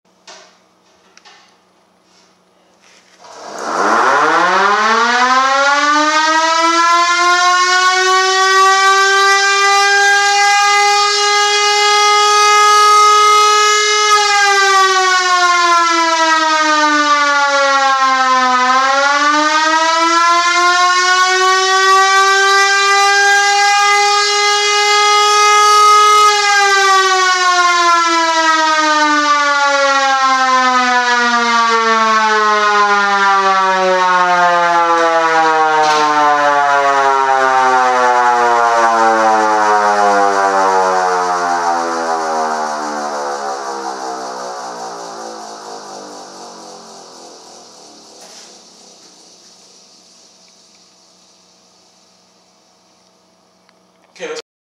Sterling Little Giant Siren
Sterling Little Giant siren running up in my garage. It runs on 240V single phase and has double 12-port rotors. The siren was running on 120V via an outlet in my garage. It was made sometime in the 1920s and still runs great!